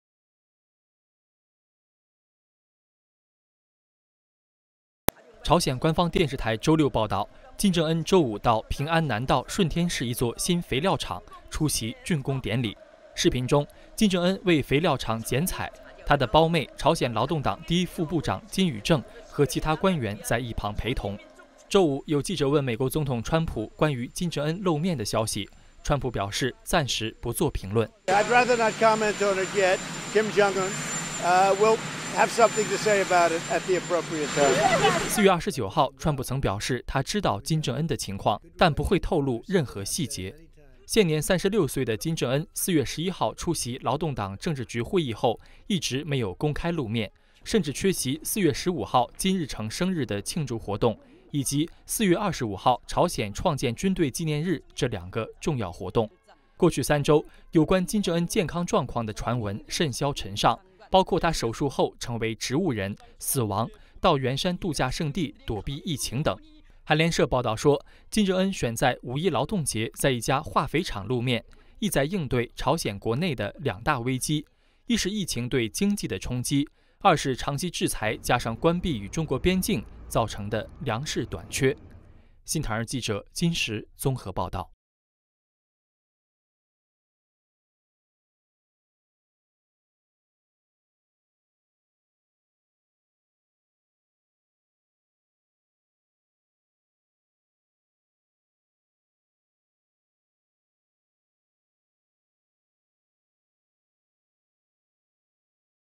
（字幕）金正恩消失20天後現身 特朗普：暫不評論
金正恩消失20天後現身 特朗普：暫不評論

發佈日期：2020年5月3日
責任編輯：Bowen